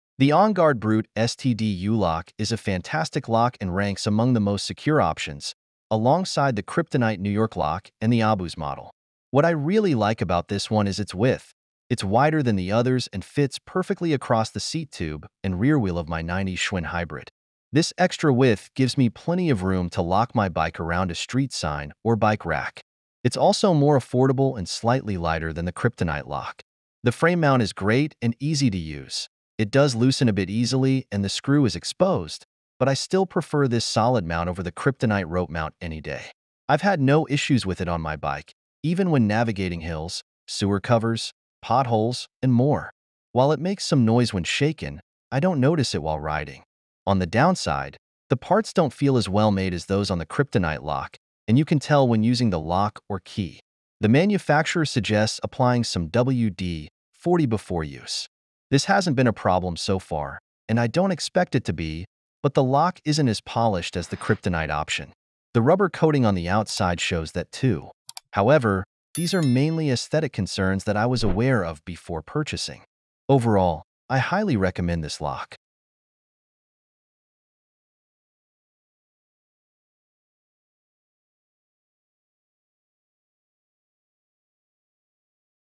Unlock the Secret: Why This U-Lock Is Better Than Kryptonite
OnGuard Brute STD U Lock

Top 5 Bike Locks
Kryptonite New York Fahgettaboudit U-Lock
Abus Granit XPlus 540 U-Lock
Hiplok Gold Wearable Chain Lock
Kryptonite Evolution Mini Bike Chain Lock

Discover why the OnGuard Brute STD U-Lock stands out among bike locks. With its wider design, affordability, and solid frame mount, it rivals top brands like Kryptonite. Despite some minor aesthetic flaws, it delivers top-notch security and performance. Watch to learn why this lock might be the best choice for your bike!

00:00 Top Security and Ideal Fit
00:26 User-Friendly Frame Mount
00:46 Minor Aesthetic Issues
01:12 Highly Recommended